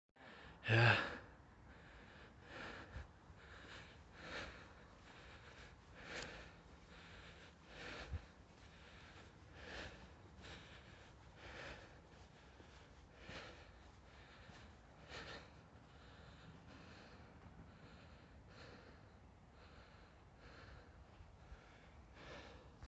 Sunrise at Church of Saint Martha-on-the-Hill, Guildford
Caught the sunrise! Morning run to St Martha's Church outside Guildford, Surrey (England, UK), near Pewley Down. On the old Pilgrim's Way and can only be accessed by foot.

Apologies for the breathing. A bit too much Reality TV! 😋